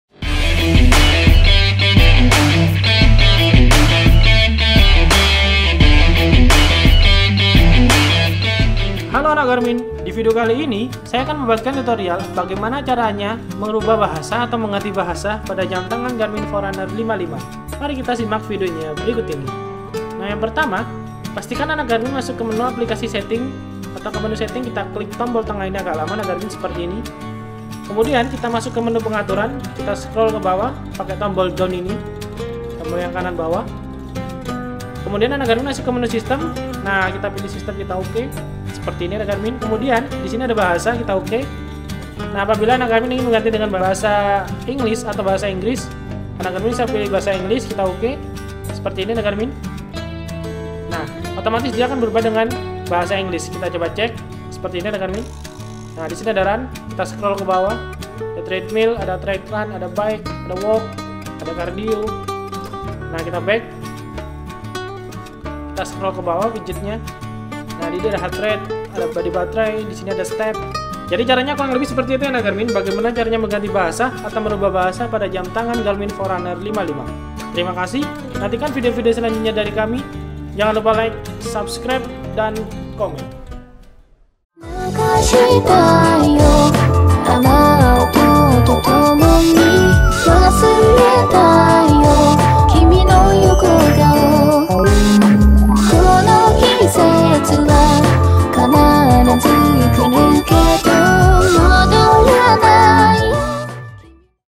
Garmin Forerunner 55 - Cara Mengganti Bahasa di Jam Garmin Forerunner 55
Ingin mengganti bahasa di jam tangan garmin anda?
Nah, ini video tutorial cara setting ganti bahasa di jam Garmin Forerunner 55.

Semoga Bermanfaat Tutorial Ini Bagi yang Membutuhkan :)